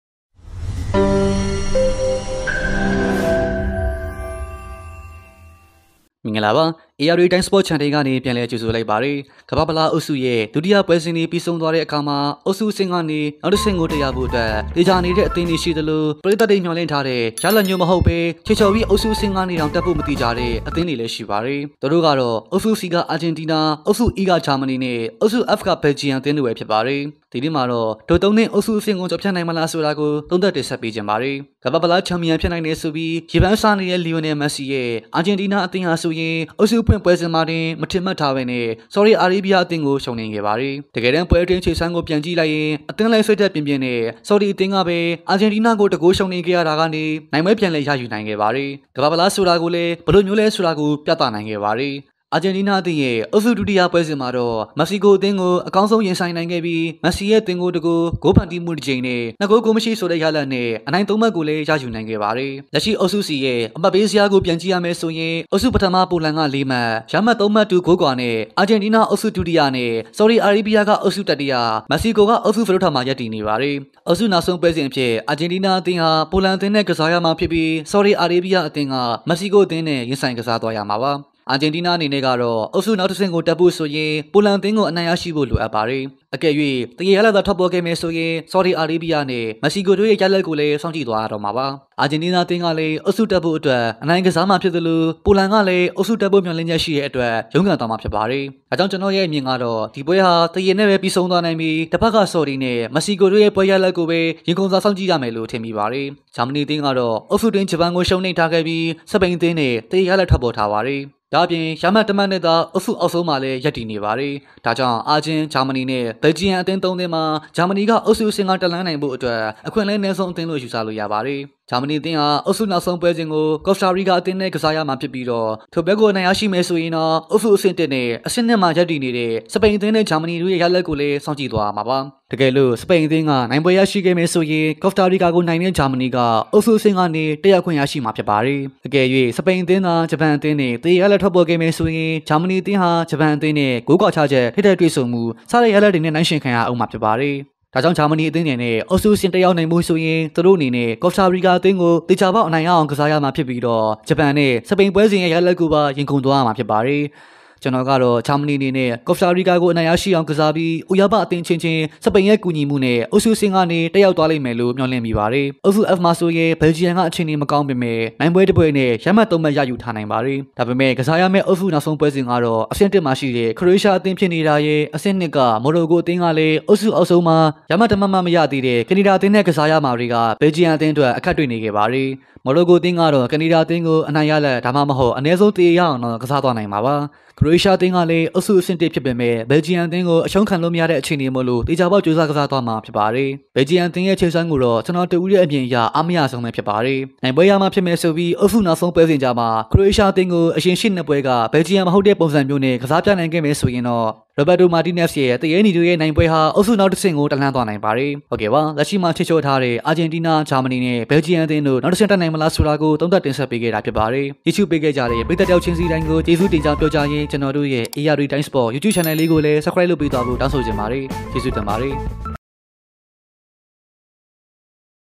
နာမည်ကြီး အာဂျင်တီးနား၊ ဂျာမနီ၊ ဘယ်လ်ဂျီယံတို့ အုပ်စုအဆင့်က ကျော်ဖြတ်နိုင်ပါ့မလား
ကမ္ဘာ့ဖလားအုပ်စု ဒုတိယပွဲစဉ်တွေ ပြီးဆုံးသွားတဲ့အခါမှာ အုပ်စုအဆင့်ကနေ နောက်တစ်ဆင့်ကို တက်ရောက်ဖို့ သေချာနေတဲ့ အသင်းတွေ ရှိသလို ပရိတ်သတ်တွေ မျှော်လင့်ထားတဲ့ ရလဒ်မျိုး မဟုတ်ပဲ ခြေချော်ကာ အုပ်စုအဆင့်ကတောင် တက်ဖို့ မသေချာနေတဲ့ အသင်းကြီးတွေ ရှိနေပါတယ်။

သူတို့ကတော့ အုပ်စု C က အာဂျင်တီးနား၊ အုပ်စု E က ဂျာမနီ နဲ့ အုပ်စု F က ဘယ်လ်ဂျီယံ အသင်းတို့ပဲ ဖြစ်ပါတယ်။ ဒီနေ့မှာတော့ အဲ့ဒီ ၃ သင်း အုပ်စုအဆင့်ကို ကျော်ဖြတ်နိုင်မလားဆိုတာကို သုံးသပ်တင်ဆက်ပေးသွားမှာ ဖြစ်ပါတယ်။